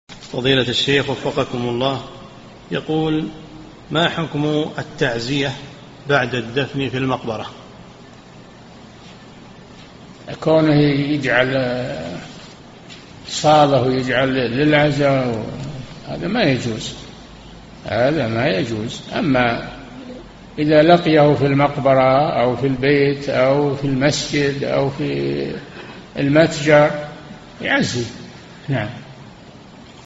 [945 -3022] ما حكم التعزية بعد الدفن في المقبرة؟ - الشيخ صالح الفوزان
-"[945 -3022] السؤال: ما حكم التعزية بعد الدفن في المقبرة؟
الجَوَابُ: كونه يجعل صالة، ويجعل للعزاء، هذا ما يجوز، هذا ما يجوز، أما إذا لقيه في المقبرة، أو في البيت، أو في المسجد، أو في المتجر، يعزي، نعم.
للاشتراك في قناة الشيخ صالح الفوزان

للاستفسار والإقتراحات والدعم
رئيس الفريق
لمشروع كبار العلماء